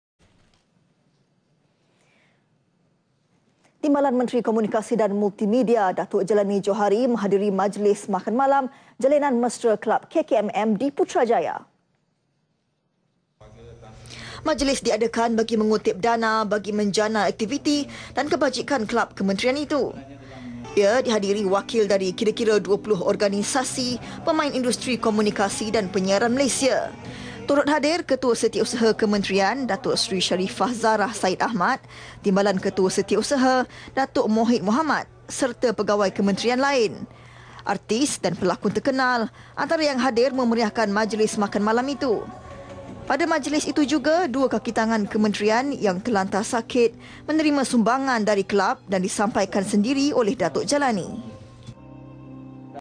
KKMM: 20 INDUSTRI KOMUNIKASI DAN PENYIARAN HADIR JALINAN MESRA KELAB KKMM [8 DIS 2016]
Timbalan Menteri Komunikasi dan Multimedia, Datuk Jailani Johari menghadiri majlis makan malam Jalinan Mesra Kelab KKMM di Putrajaya.